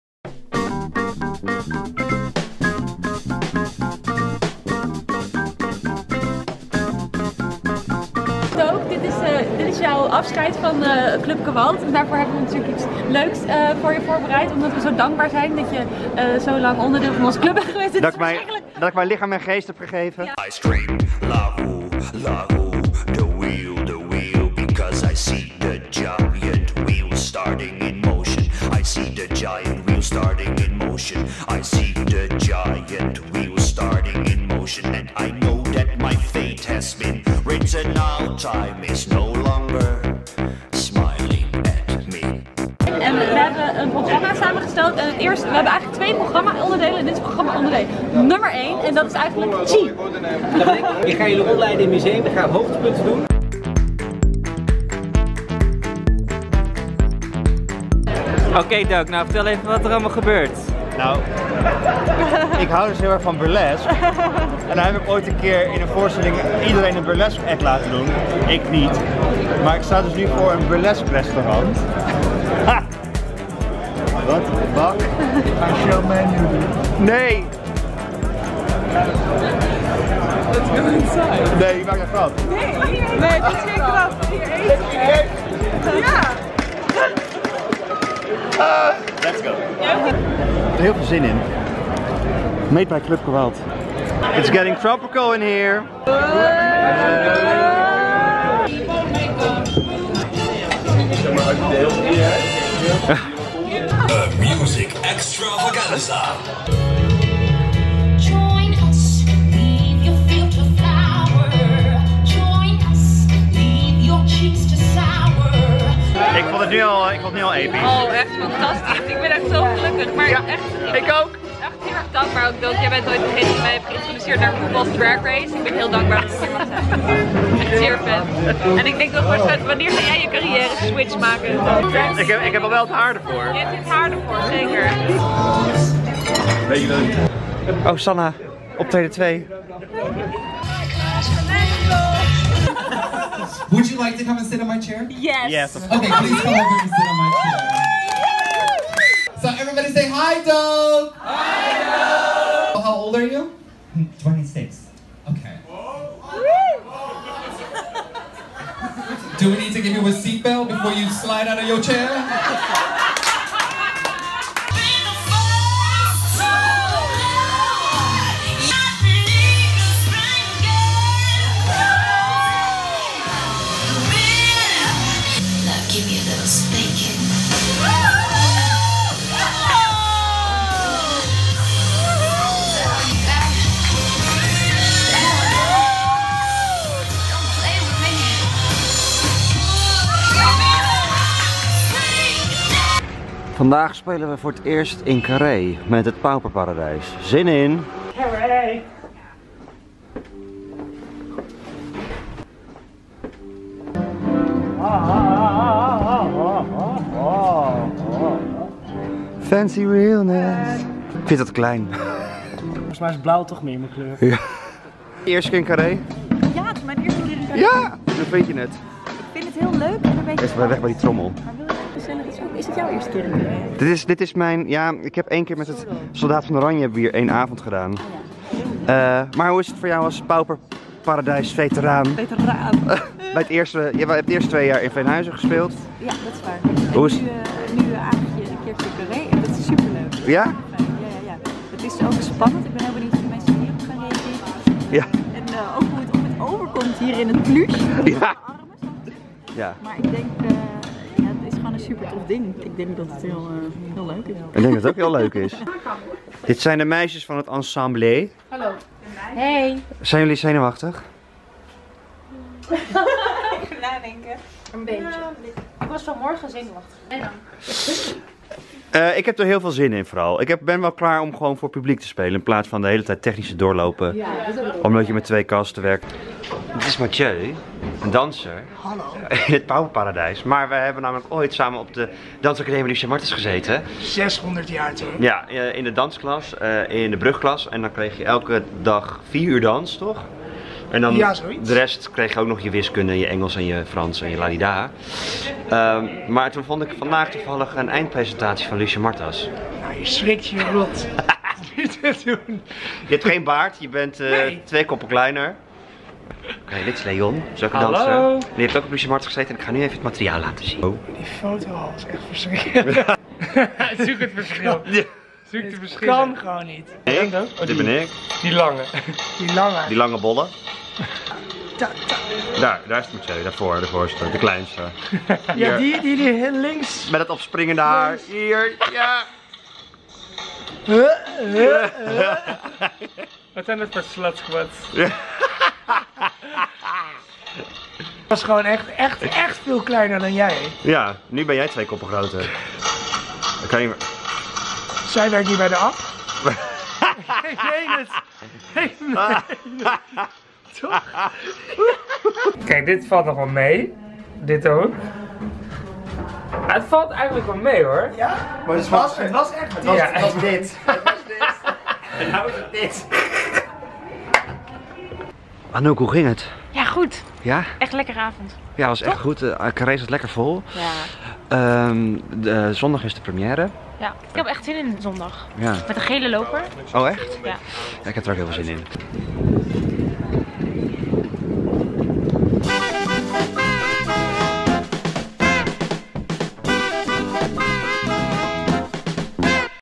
LAPDANCE VAN DRAGQUEEN!! | DOOK #46
Ik krijg een afscheid van CLUB GEWALT, het muziektheater-collectief waar ik vier jaar voorstellingen mee heb gemaakt/gespeeld/geproduceerd. Incl een lapdance!! En we spelen voor het eerst met Het Pauperparadijs in Carre!